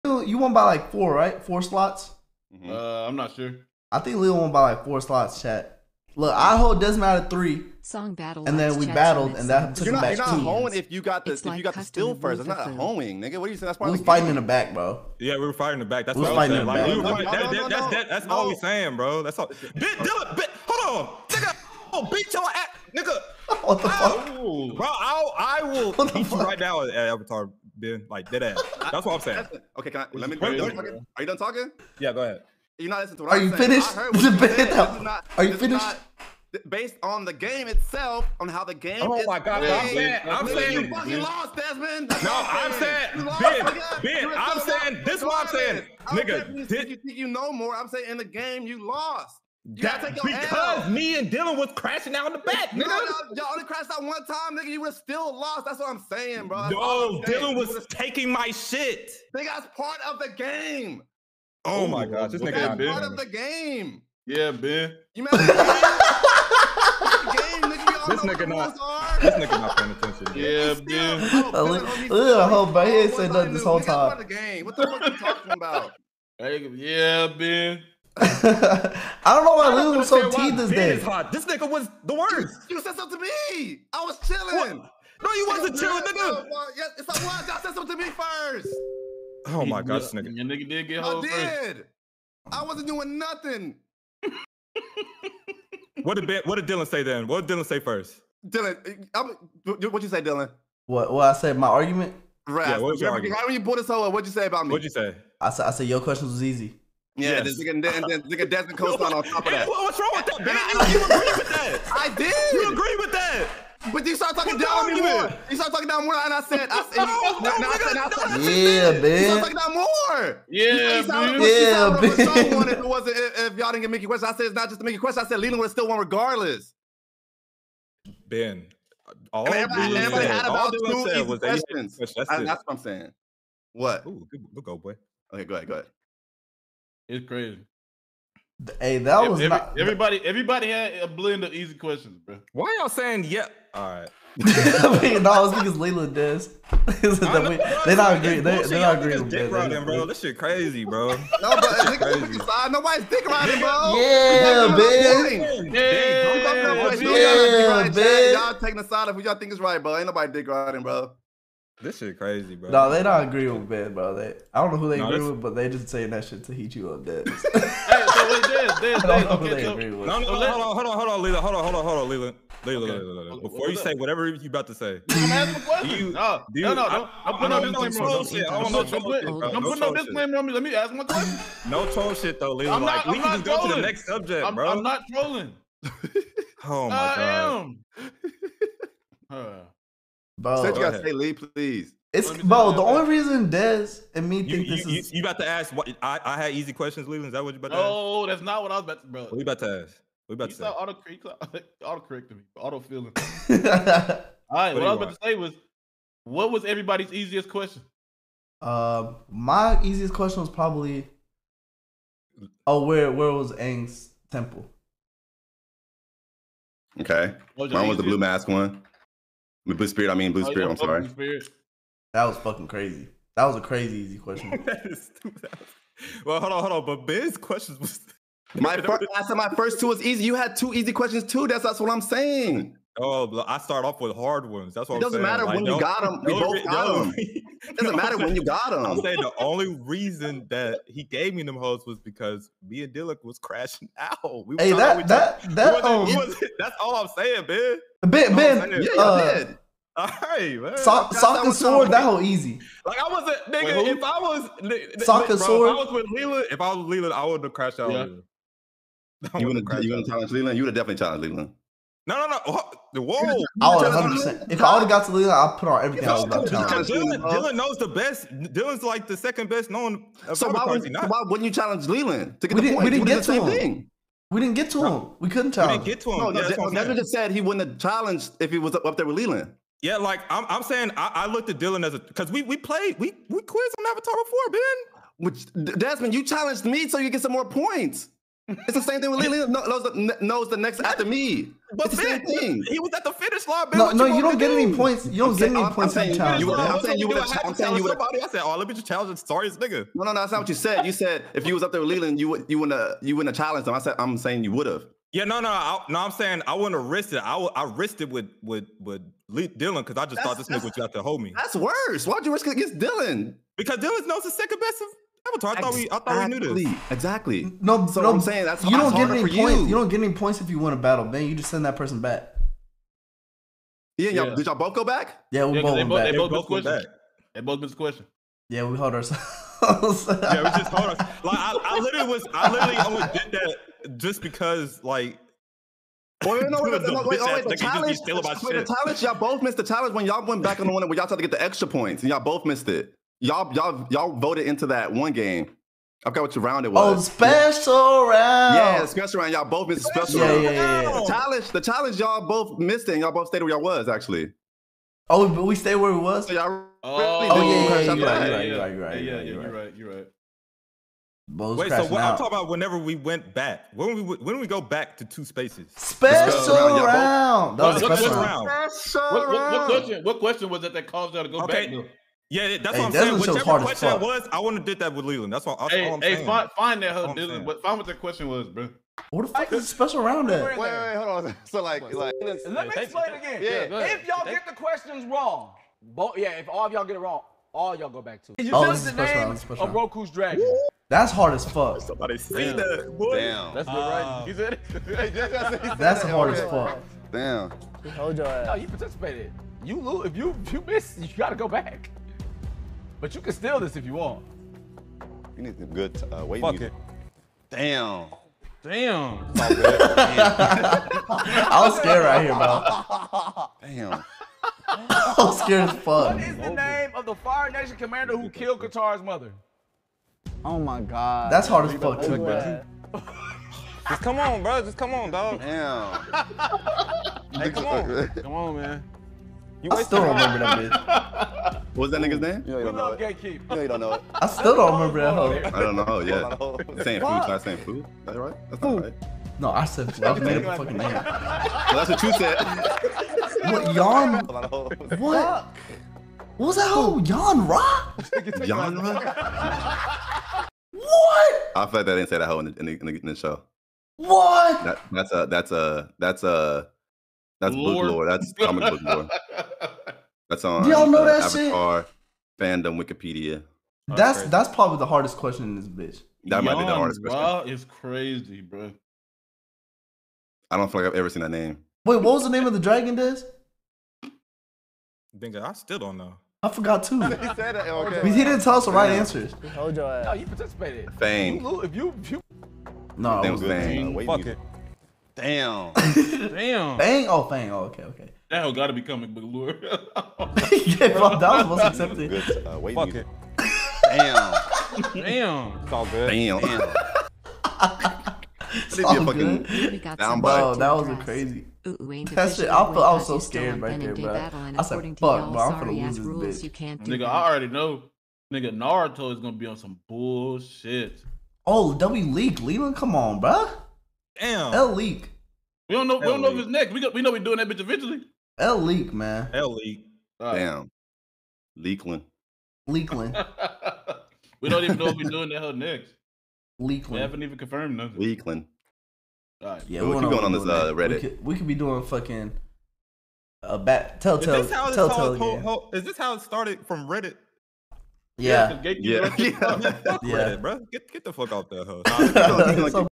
Rdc Is Still Heated About Gatekeep 😂
Desmond is still crashing out about the events of Gatekeep by Rdcworld1